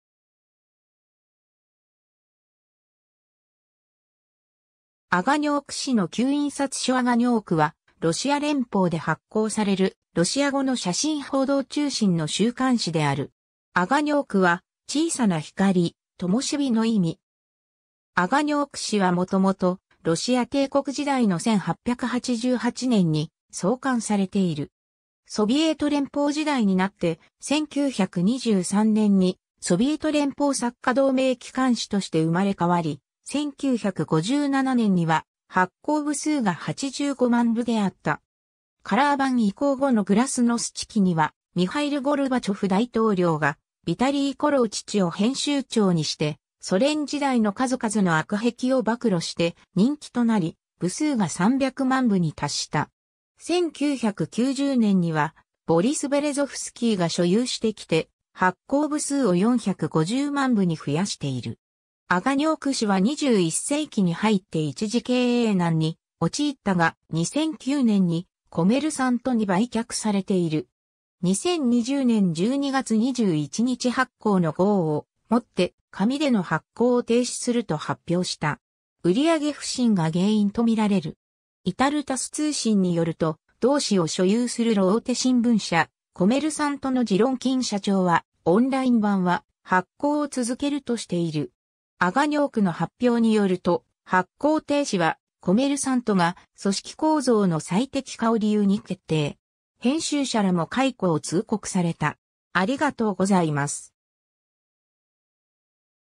アガニョーク
アガニョーク誌の旧印刷所（設計：エル・リシツキー） アガニョーク（ロシア語: Огонёк）は、ロシア連邦で発行されるロシア語の写真報道中心の週刊誌である。
アガニョークは「小さな光、ともしび」の意味。
 アガニョーク誌はもともとロシア帝国時代の1888年に創刊されている。
ソビエト連邦時代になって、1923年にソビエト連邦作家同盟機関誌として生まれ変わり、1957年には発行部数が85万部であった。
 カラー版移行後のグラスノスチ（情報公開）期にはミハイル・ゴルバチョフ大統領がヴィタリー・コローチチを編集長にして、ソ連時代の数々の悪癖を暴露して人気となり、部数が300万部に達した。
1990年にはボリス・ベレゾフスキーが所有してきて、発行部数を450万部に増やしている。
 アガニョーク誌は21世紀に入って一時経営難に陥ったが、2009年にコメルサントに売却されている。
 2020年12月21日発行の号をもって紙での発行を停止すると発表した。
売り上げ不振が原因とみられる。
イタル・タス通信によると、同誌を所有する露大手新聞社「コメルサント」のジロンキン社長は「オンライン版は発行を続ける」としている。
アガニョークの発表によると、発行停止はコメルサントが「組織構造の最適化」を理由に決定。
編集者らも解雇を通告された。